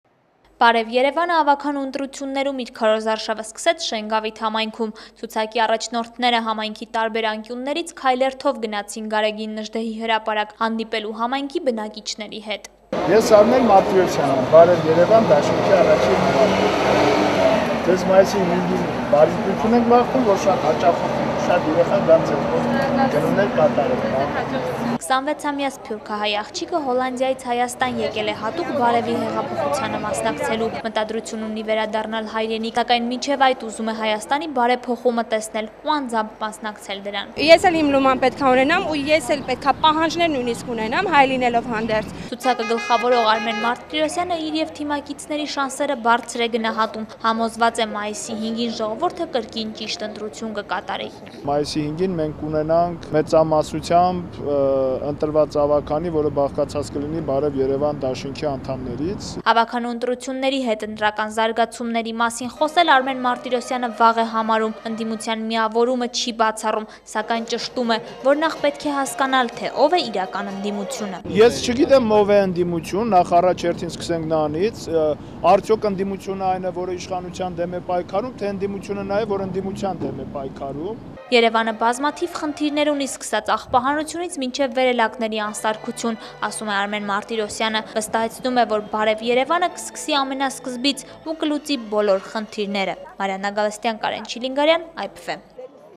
«Բարեւ Երեւանը» սկսեց
«Բարեւ Երեւան» դաշինքը ավագանու ընտրություններում իր քարոզարշավը սկսեց Շենգավիթ համայնքում: Ցուցակի առաջնորդները, համայնքի տարբեր անկյուններից, քայլերթով գնացին Գարեգին Նժդեհի հրապարակ հանդիպելու համայնքի բնակիչների հետ: